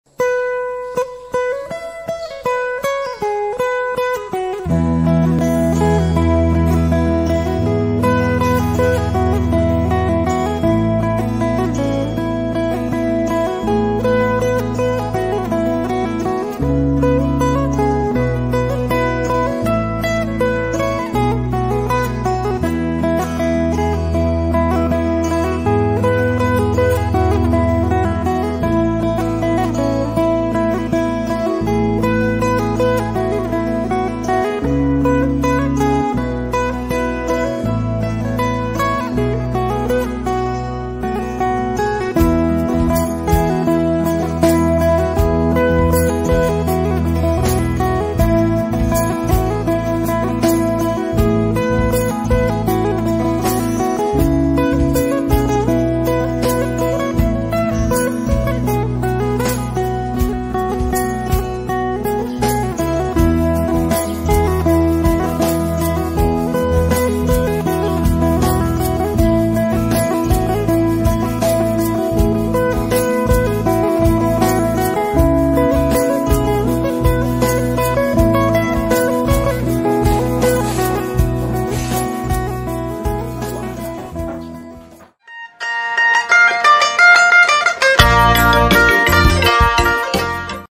Baing Biuk Kenyah | Parang Besar
Baing by the kenyah people.
Traditional weapon of the Dayak people of Borneo.

Used by: Dayak (Sea Dayak, Bidayuh, Kayan, Kenyah, Penan, Lun Bawang/Lundayeh)

Scabbard/sheath: Wood, antler or bone fittings, animal or human hair